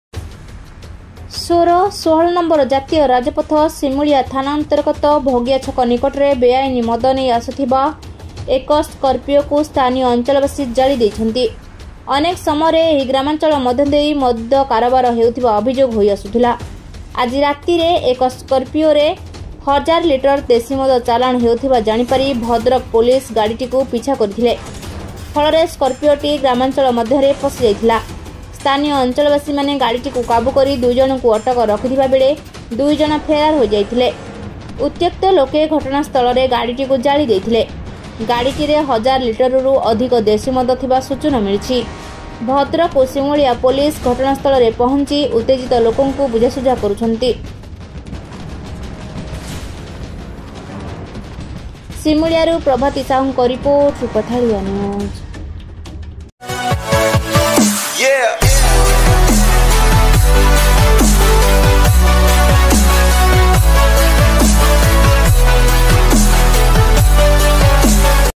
ଜାଳିଦେଲେ ସ୍କର୍ପିଓ କୁ...ବେଆଇନ ମଦ ନେଇ ଆସୁଥିବା ବେଳେ ...
ଜାଳିଦେଲେ ସ୍କର୍ପିଓ...ବେଆଇନ ମଦ ନେଇ ଆସୁଥିବା ବେଳେ ...